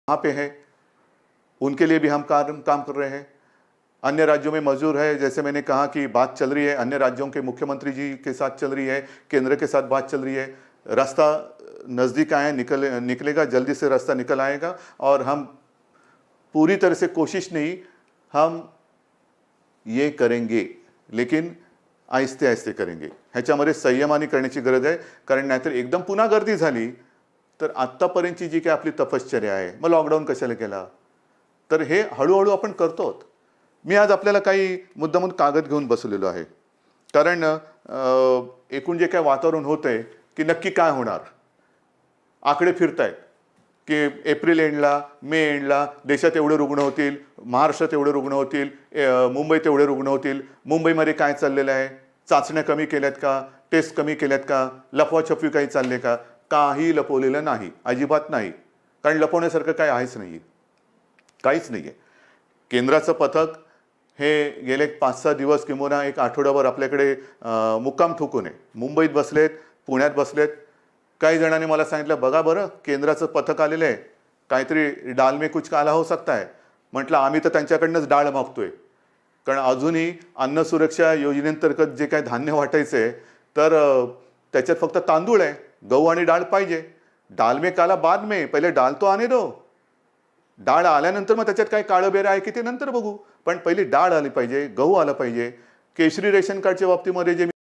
covid19-CM. Uddhav Thackeray said that everyone should reach their city, but do not gather the crowd
Chief Minister Uddhav Thackeray said that everyone should reach their city, but do not gather the crowd